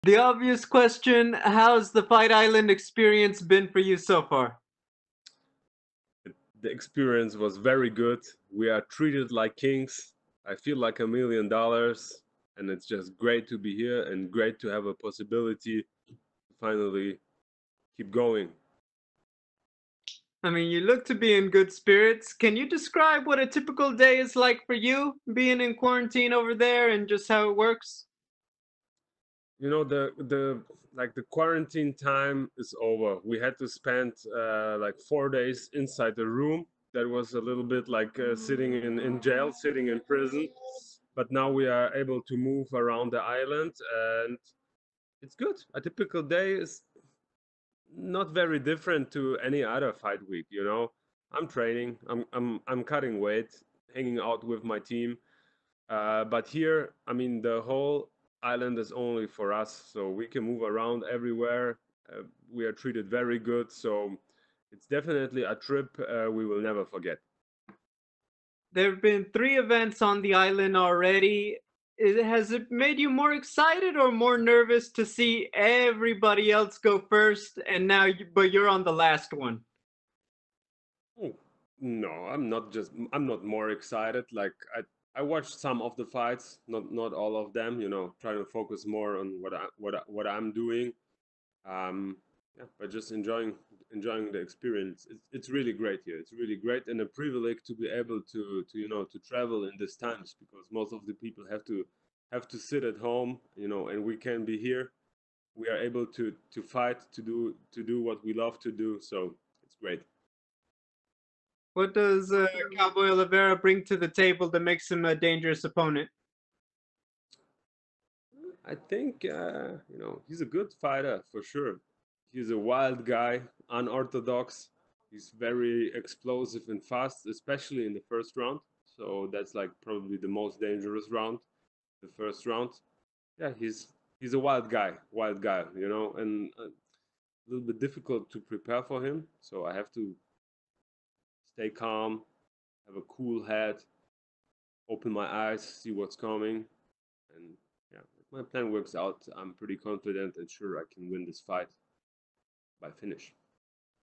UFC Fight Island 3: Peter Sobotta Talks Fight with Cowboy Oliveira
Peter Sobotta discussed his Fight Island experience and facing Cowboy Oliveira ahead of UFC Fight Island 3.